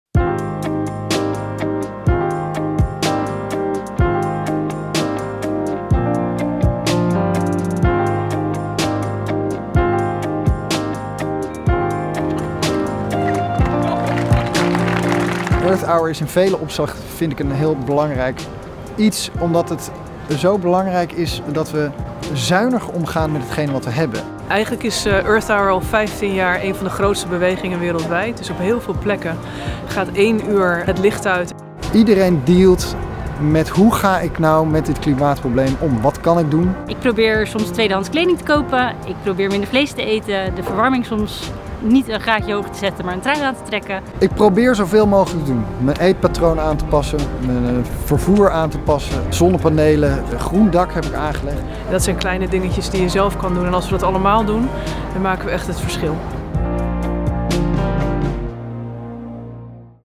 WWF Earth Hour 2022: één uur het licht uit voor de aarde
Zaterdag 26 maart deden wereldwijd weer miljoenen mensen, bedrijven en gemeenten één uur het licht uit tijdens Earth Hour. Van 20.30 tot 21.30 uur lokale tijd werd daarmee een krachtig signaal afgegeven voor natuur en het klimaat. In deze uitdagende tijden biedt Earth Hour een moment van solidariteit om gezamenlijk stil te staan bij een duurzame en veilige toekomst voor iedereen. In Nederland was het switch off-moment in Rotterdam waar het wereldberoemde Depot Boijmans Van Beuningen op zwart ging. Pianist Ruben Hein speelde een muzikaal stuk en precies om 20:30 hield de muziek op en ging het licht van het Depot Boijmans Van Beuningen in Rotterdam tot 21:30 uit.

“Licht uit, duurzaamheid aan”
Earth Hour is bekend van het uitzetten van het licht en een moment om iets duurzaams te doen. WWF wil mensen inspireren meer in balans met onze aarde en meer in harmonie met natuur te leven. En dat kan op verschillende manieren zoals:
- het opvangen van regenwater om kamerplanten water te geven
- de verwarming een graadje lager zetten om energie te besparen
- de was buiten drogen in plaats van altijd in de droger
- door iets vaker plantaardig te eten

Het volgende Earth Hour is zaterdag 25 maart 2023.

Of volg WWF via…